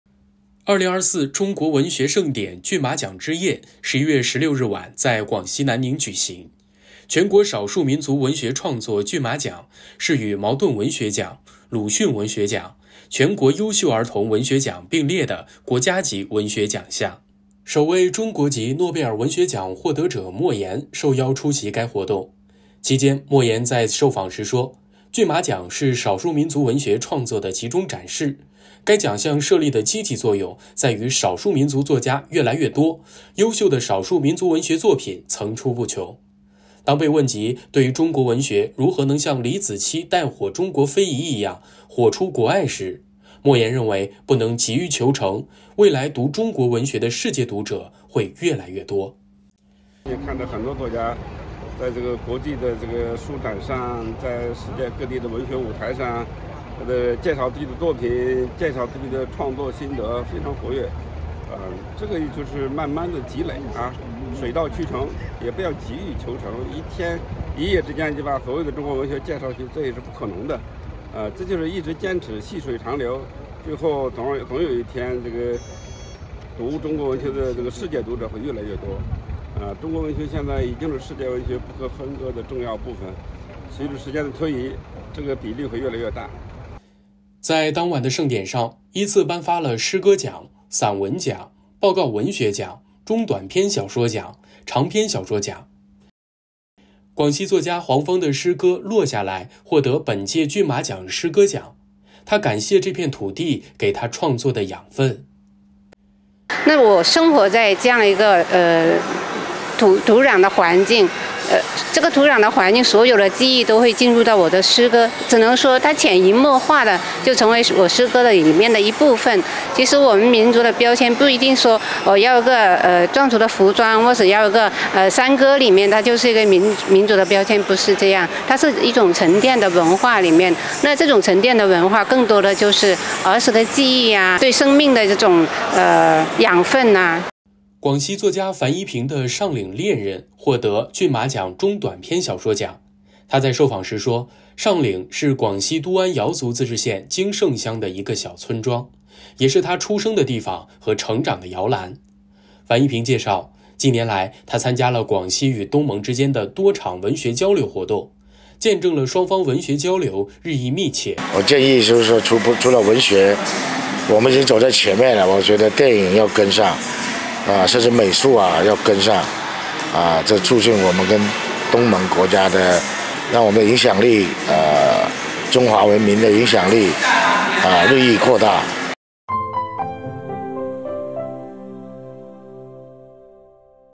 骏马奖之夜南宁举行 莫言：中国文学的世界读者会越来越多
2024中国文学盛典·骏马奖之夜11月16日晚天在广西南宁举行，全国少数民族文学创作骏马奖是与茅盾文学奖、鲁迅文学奖、全国优秀儿童文学奖并列的国家级文学奖项。

首位中国籍诺贝尔文学奖获得者莫言受邀出席该活动。其间，莫言在受访时说，骏马奖是少数民族文学创作的集中展示，该奖项设立的积极作用在于少数民族作家越来越多、优秀的少数民族文学作品层出不穷。被问及对于中国文学如何能像李子柒带火中国非遗一样时，莫言称，不能急于求成，未来读中国文学的世界读者会越来越多。